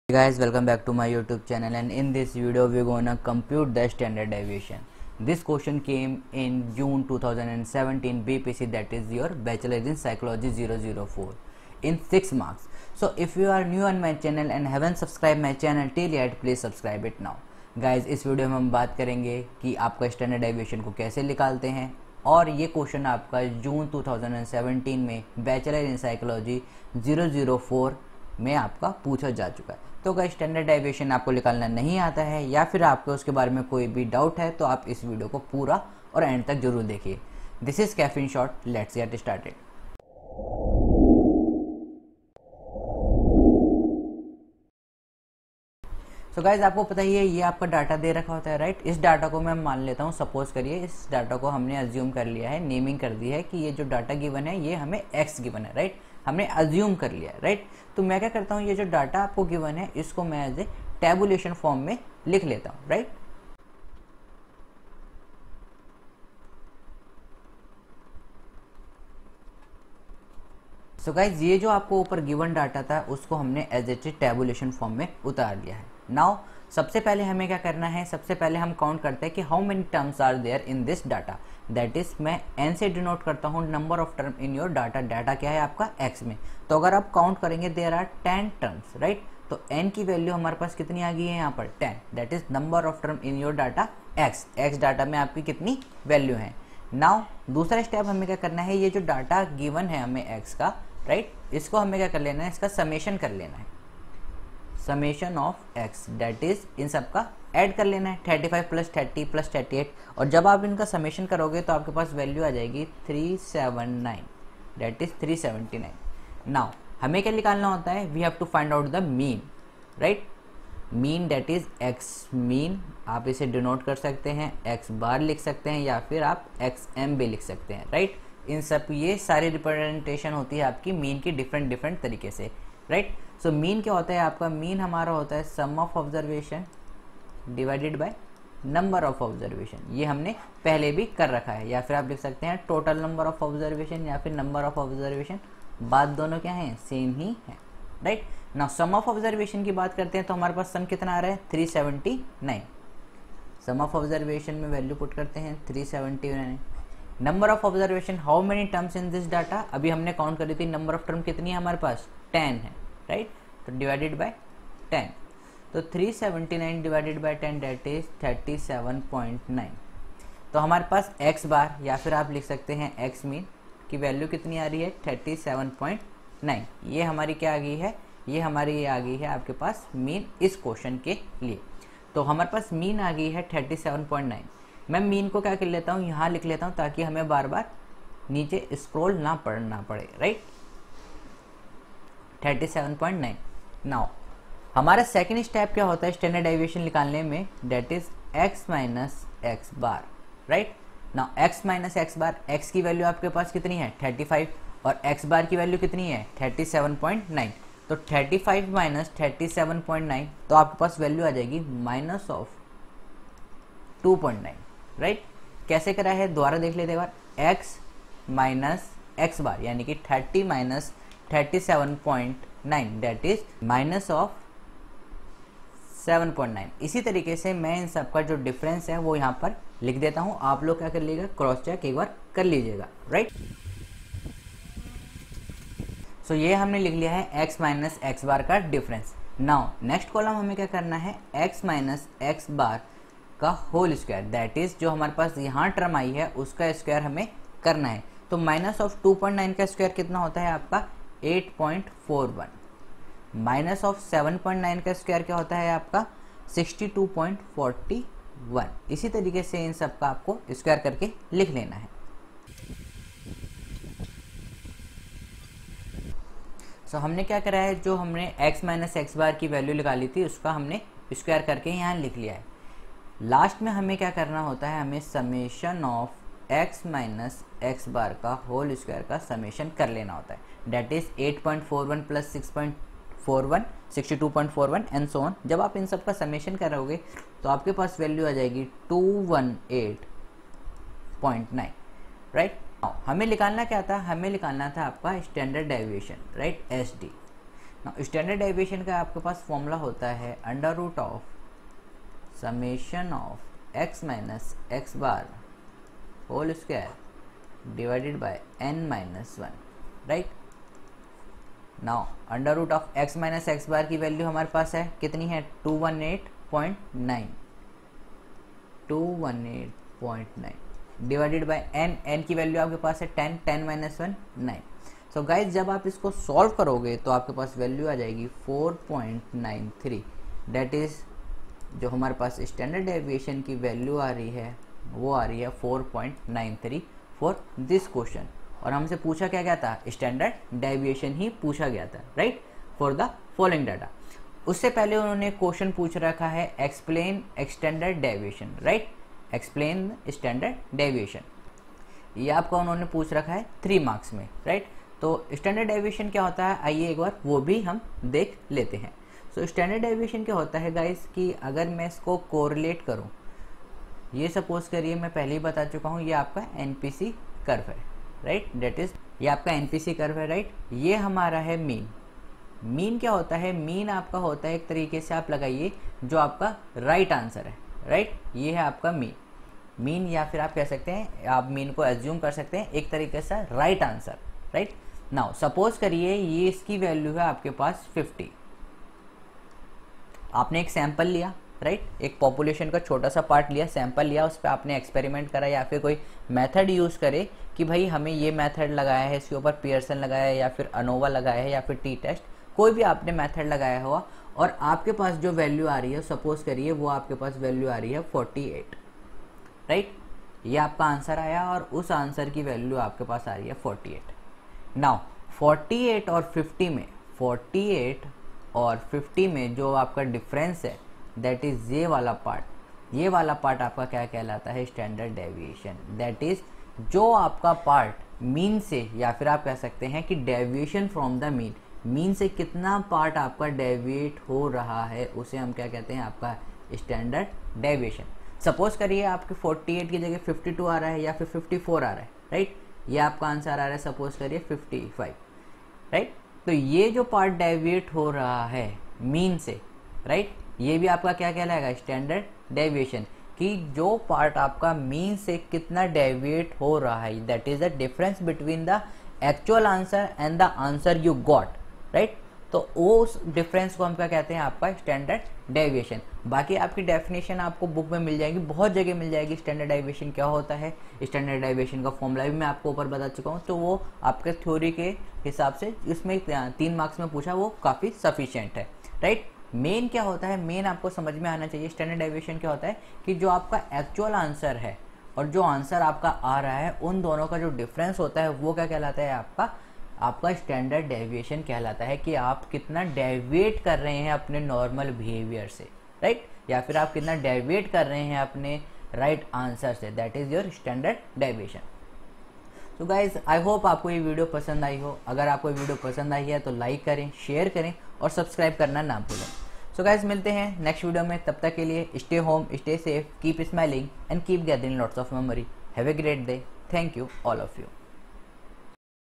How to calculate STANDARD DEVIATION // previous year questions(JUNE 2017)
Standard Deviation

The Standard Deviation is a measure of how spread out numbers are.

Its symbol is σ (the greek letter sigma)

The formula is easy: it is the square root of the Variance. So now you ask, "What is the Variance?"
Variance

The Variance is defined as:

The average of the squared differences from the Mean.